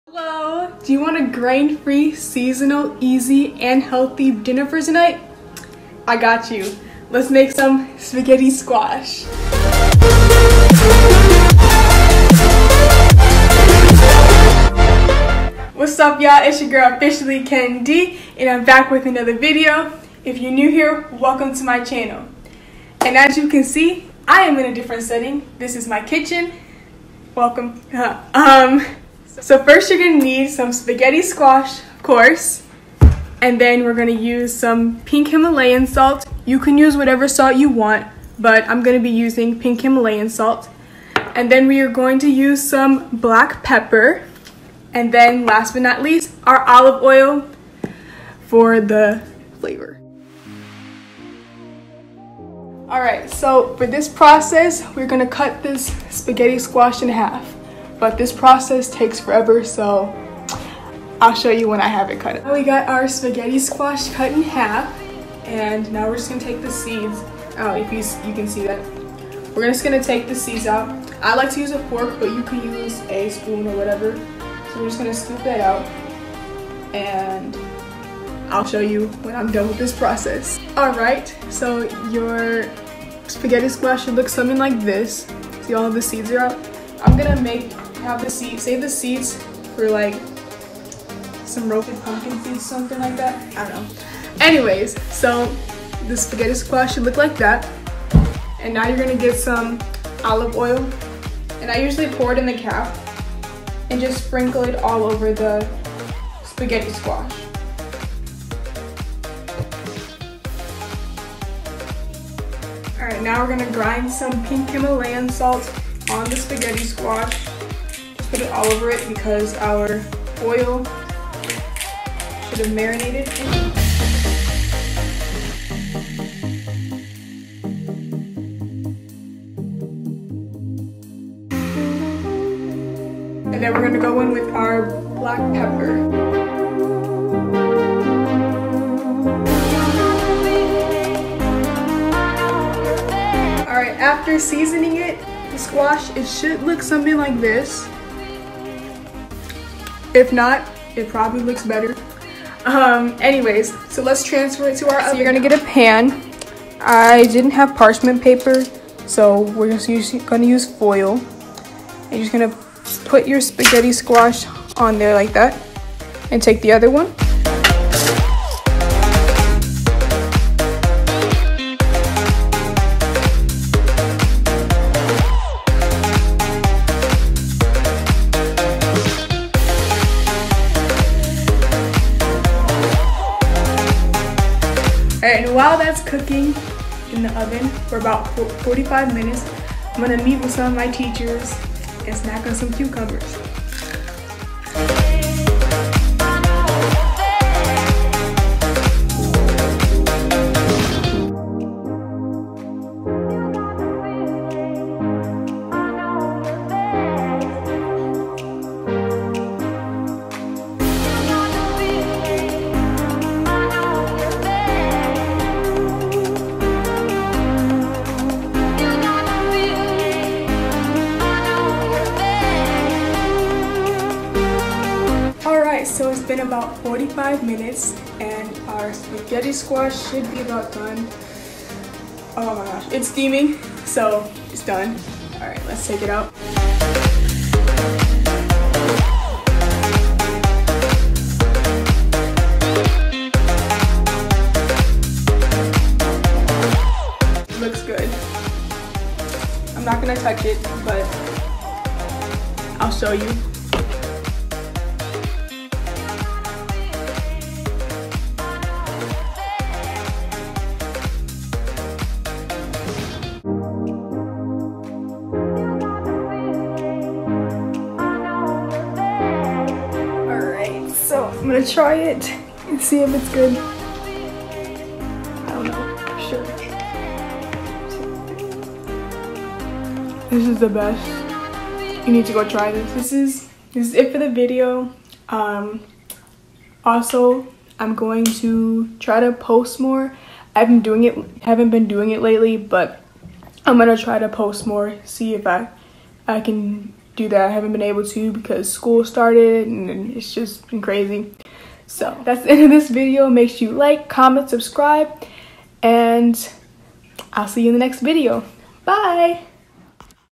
How to Cook Spaghetti Squash
Hellooo everyone,

This was my second time trying Spaghetti squash. I hope that you try it and love it as much as I do.

In the future I will try to start posting more often when I have the chance. Let me know if you want to see any other videos in the comments down below.

Kennedy🤍